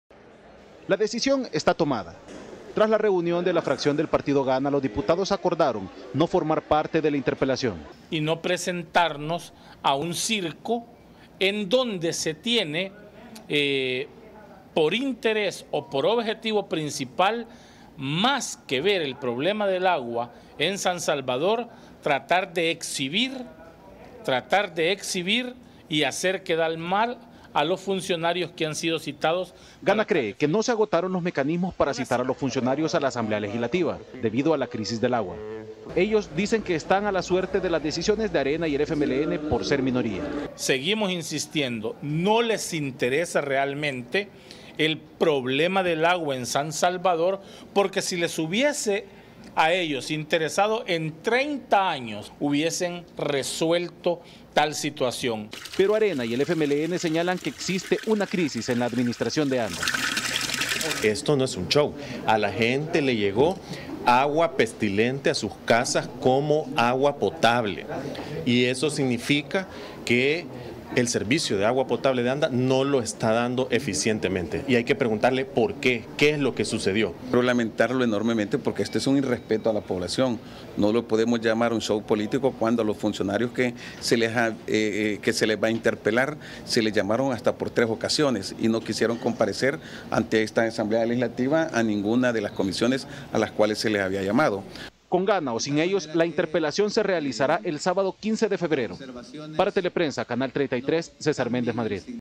Teleprensa33 | GANA considera la interpelación un "show político"
El partido Gran Alianza por la Unidad Nacional" (GANA) aseguró que no participará en la interpelación de la ministra de salud y del presidente de anda, por considerarlo un "show político".
Los diputados dijeron que la crisis del agua es un problema heredado de las administraciones anteriores.

Martes 4 de febrero de 2020